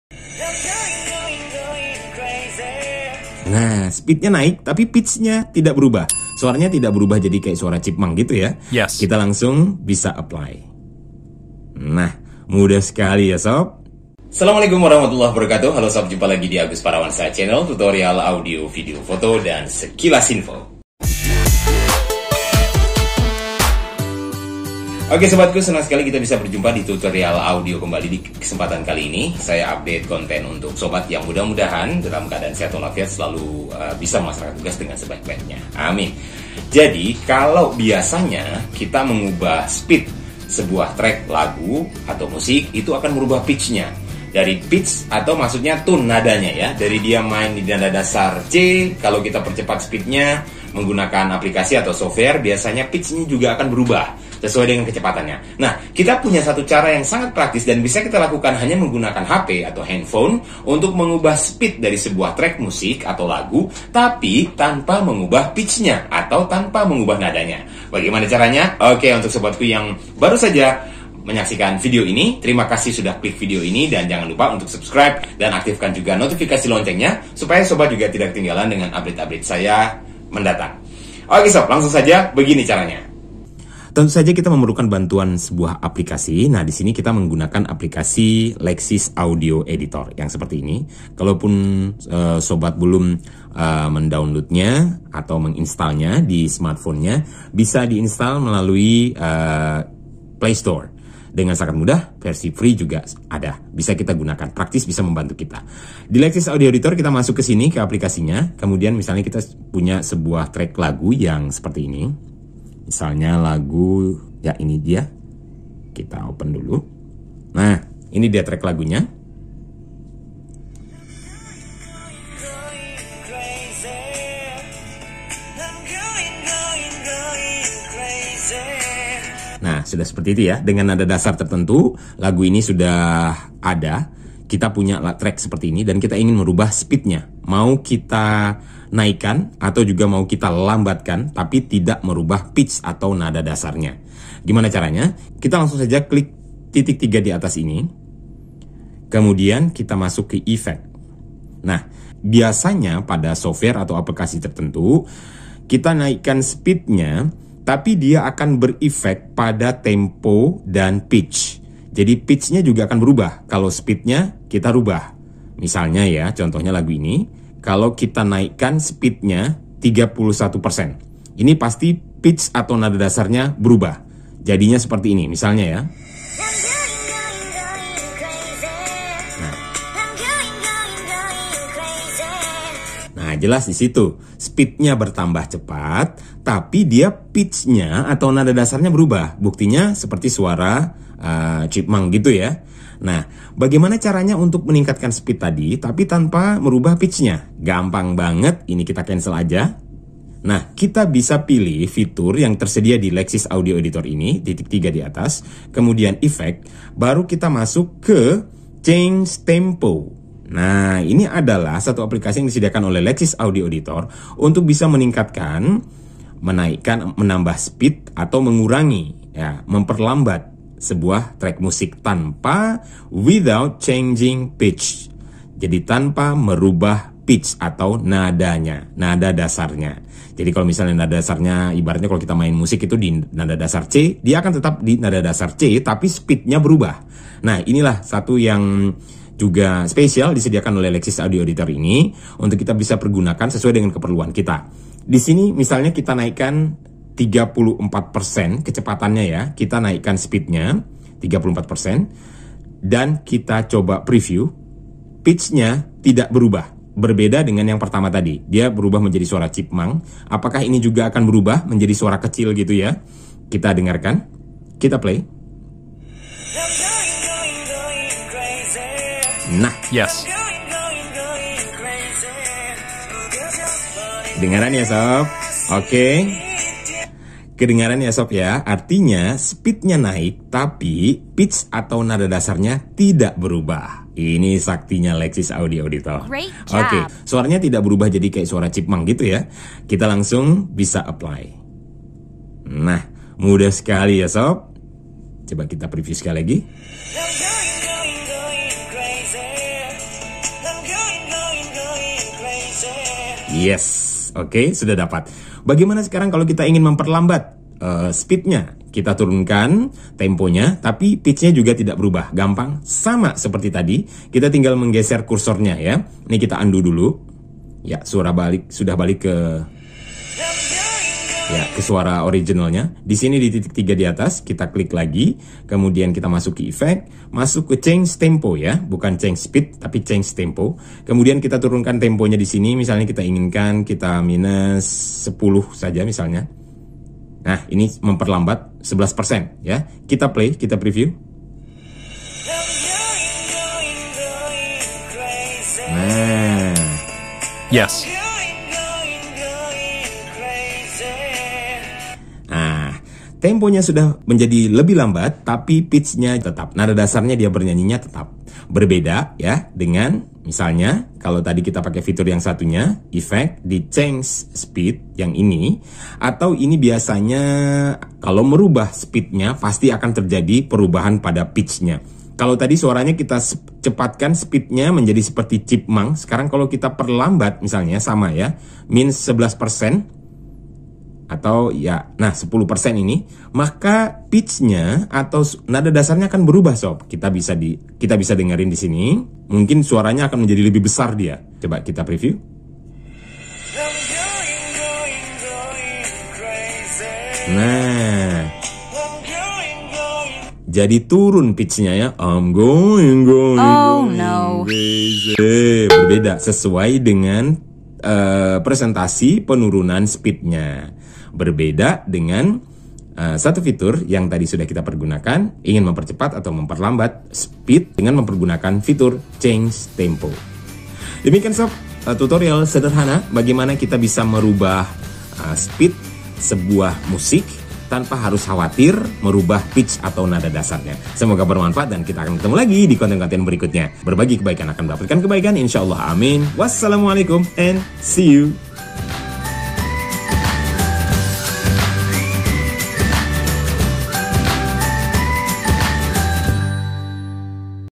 Cara Mengubah Speed Musik Tapi Pitch (nada dasarnya) Tidak Ikut Terubah Dengan Lexis Audio Editor
Untuk mengubah speed atau tempo sebuah clip audio caranya simpel banget. Lewat hp android kita sudah bisa melakukannya.

Dengan bantuan aplikasi Lexis Audio Editor, satu clip audio bisa dirubah speednya dengan sangat cepat sesuai keinginan, tanpa merubah pitch (nada dasar) musik tersebut.

Ini sangat penting diketahui, mengingat pada umumnya software atau aplikasi pengubah speed akan ikut merubah pitch.

Nah kalau ingin mengedit clip audio misalnya mempercepat atau memperlambat speednya tapi pitchnya tidak ikut terubah ? Selengkapnya di video tutorial ini.

Semoga bermanfaat.

Sedangkan tentang editing audio menggunakan PC atau Laptop (pakai software Cool Edit Pro, Fruty Loops), bisa sobat intip 😊 di playlist yang ini ya